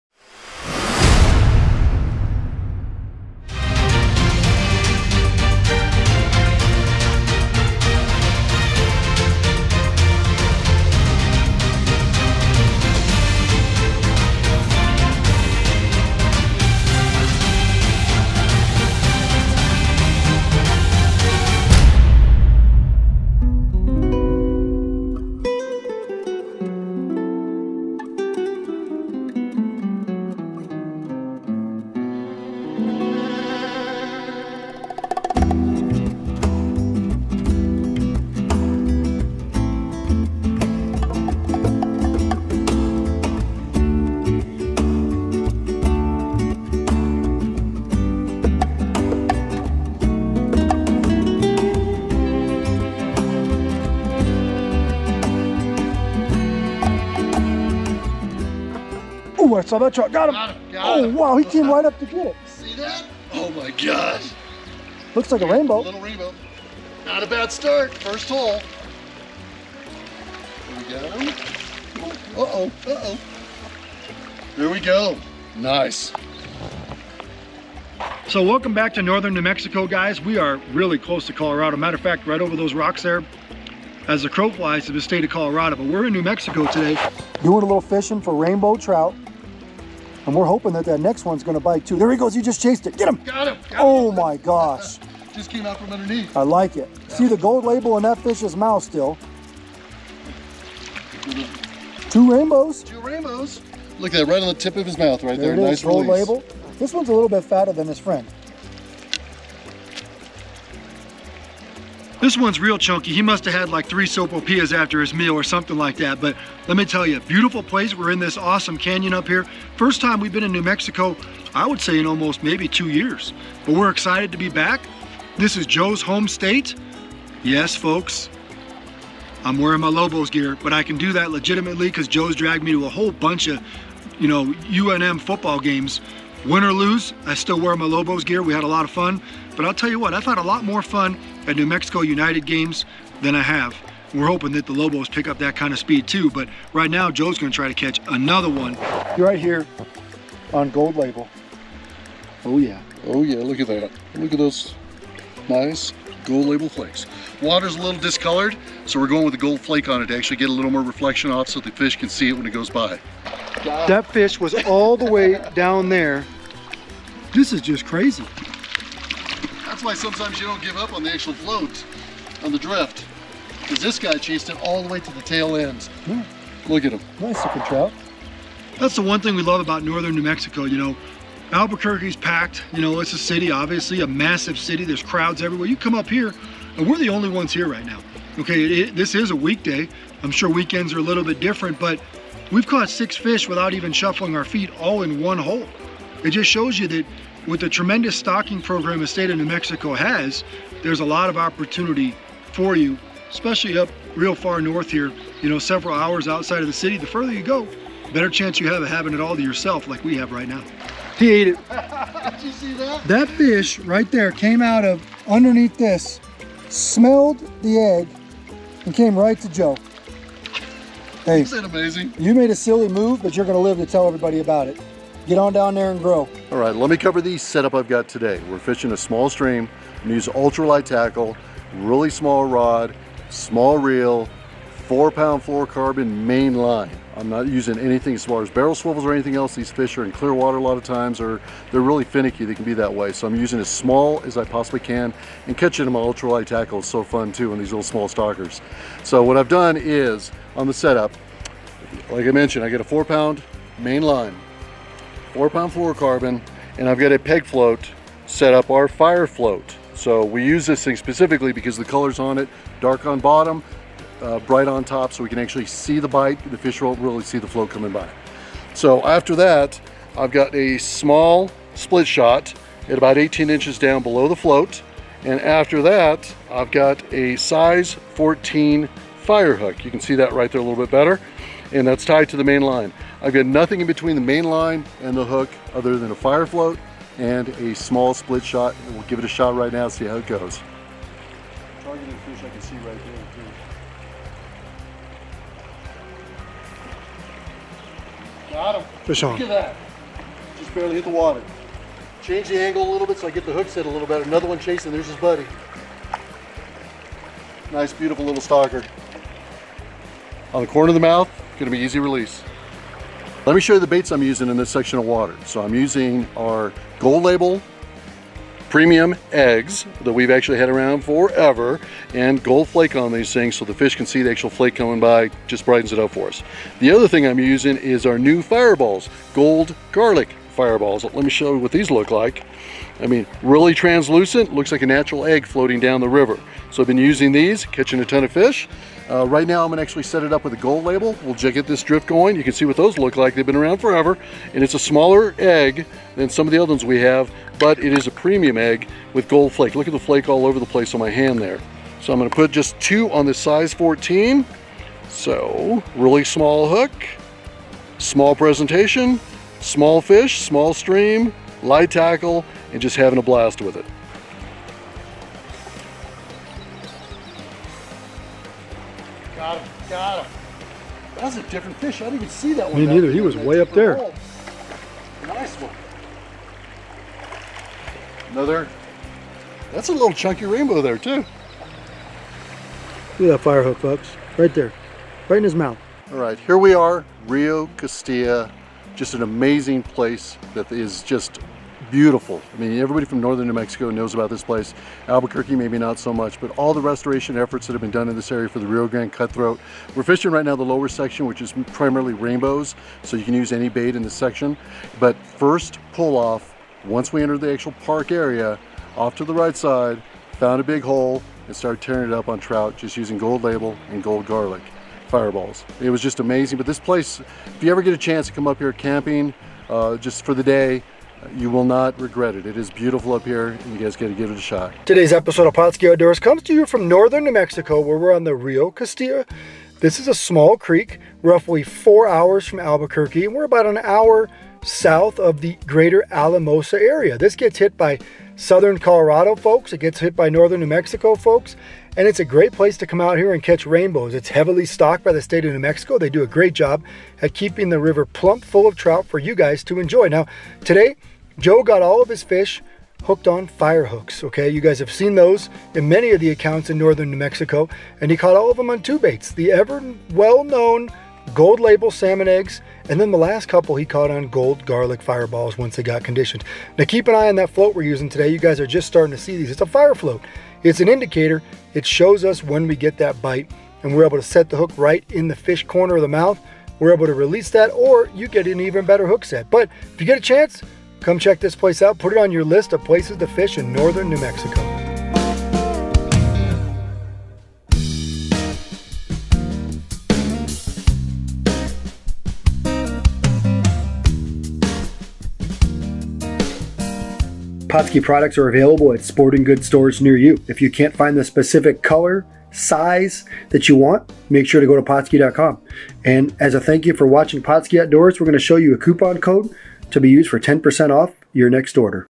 Trout Fishing New Mexico's Rio Costilla
New Mexico's Rio Costilla is the perfect place to avoid the heat and catch a limit of trout this summer in the Land of Enchantment. Find out how to catch rainbows here now in this episode of Pautzke Outdoors.